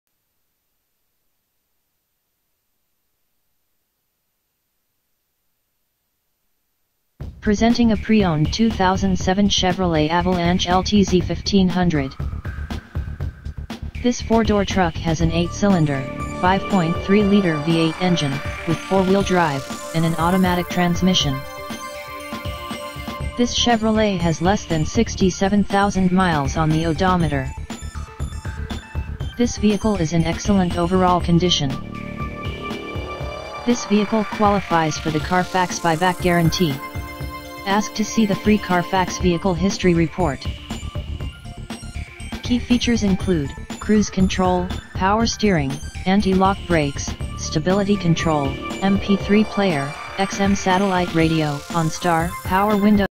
2007 Chevrolet Avalanche LTZ 1500 for sale in Lakewood, NJ
2007 CHEVROLET AVALANCHE LTZ 4X4, 5.3L FFV V8, XD 22 INCH MATTE BLACK WHEELS, NITTO GRAPPLER TIRES, LEATHER, BOSE SOUND, AM/FM CD, POWER HEATED SEATING, CLEAN CARFAX WEFINANCE!